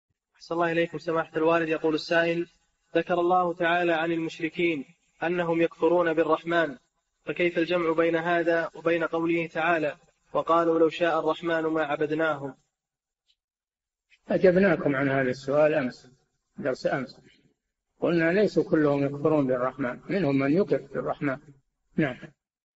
[1935- 4717] كيف الجمع بين قوله تعالى: (وهم يكفرون بالرحمن) وقوله:﴿وقالوا لو شاء الرحمن ما عبدناهم﴾؟
[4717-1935] ذكر الله تعالى عن المشركين أنهم يكفرون بالرحمن فكيف الجمع بين هذا وبين قوله تعالى: ﴿وَقَالُوا لَوْ شَاءَ الرَّحْمَنُ مَا عَبَدْنَاهُمْ﴾ [الزخرف: 20]؟
الجَوَابُ: أجبناكم عن هذا السؤال أمس في درس أمس، وقلنا ليسوا كلهم يكفرون بالرحمن، منهم من يقر بالرحمن.
السيرة الذاتية :
للاشتراك في قناة الشيخ صالح الفوزان
لمشاهدة حلقات فتاوى على الهواء كاملة

للاستفسار والإقتراحات والدعم
رئيس الفريق
لمشروع كبار العلماء